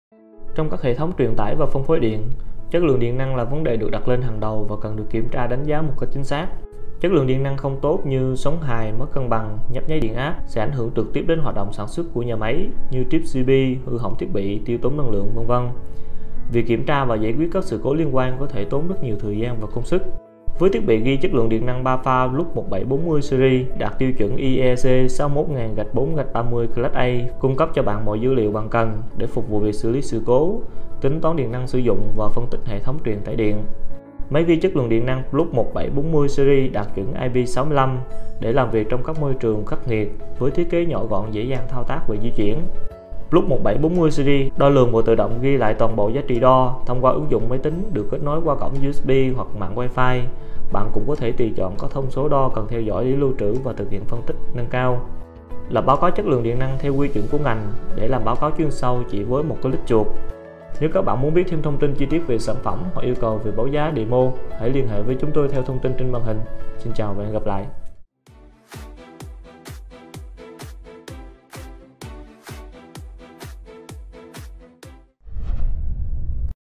Fluke 1740 Series - Ghi mọi thông số điện quan trọng trong thời gian thực
Theo dõi thông số điện để xử lý sự cố dễ dàng hơn với Fluke 1740 Series.
Tìm hiểu thêm về tính năng máy ghi điện Fluke 1740 Series tại
Yêu cầu Demo & báo giá:
VPDD Fluke tại Việt Nam
Kênh Youtube Fluke Việt Nam lên video mới mỗi tuần.